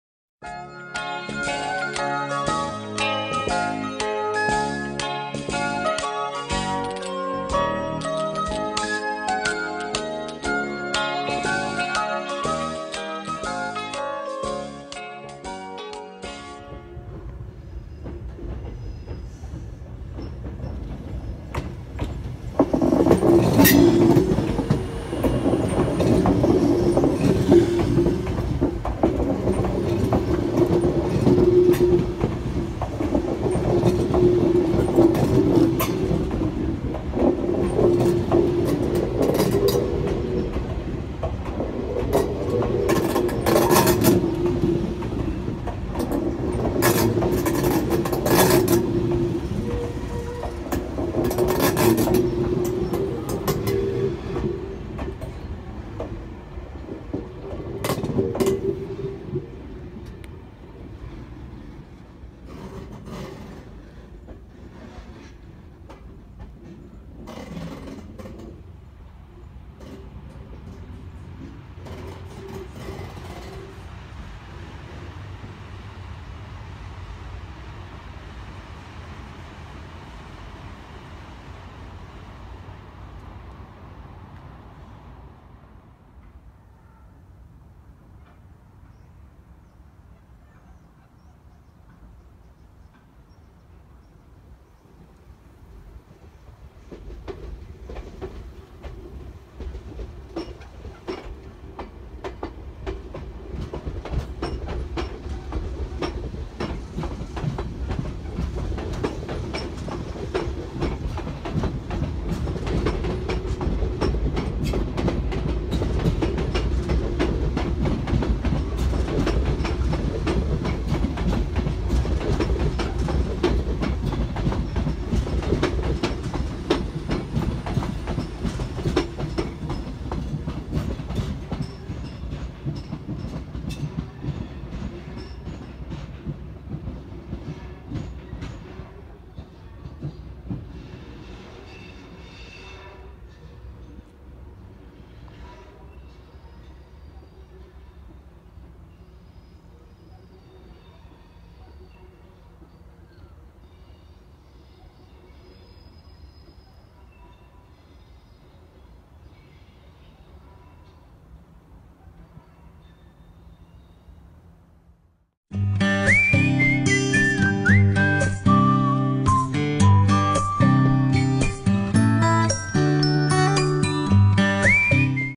【相鉄】7000系7707F(休車編成) かしわ台車両センター構内入換の様子
休車中の7000系7707F4両と新7000系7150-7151の計6両が、厚木操車場への回送準備として構内入換を行いました。

動画にはありませんが、この後モヤ700系と連結されました。